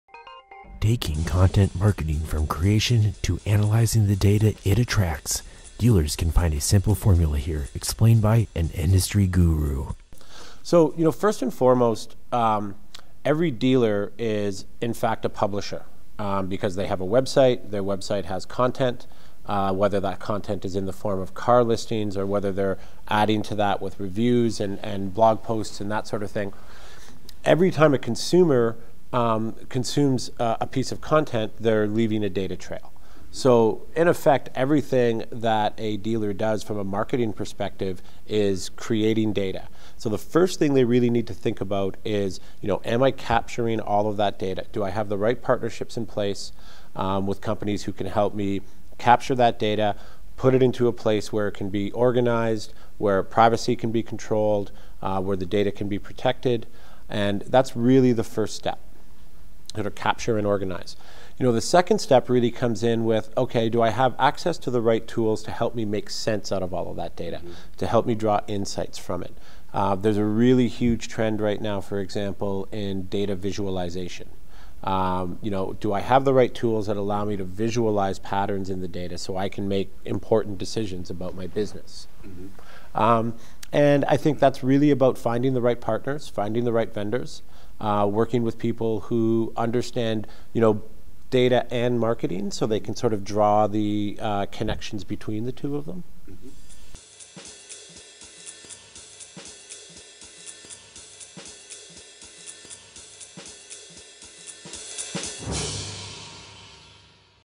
The Essential Steps of Content Marketing | James Prudhomme | AIN
Taking content marketing from creation to analyzing the data it attracts, dealers can find a simple formula here, explained by an industry guru.

Featuring
James Prudhomme
CEO, DATACRATIC

Ain_10_24_2012_DATACRATIC_Group_James_Prudhomme_VC1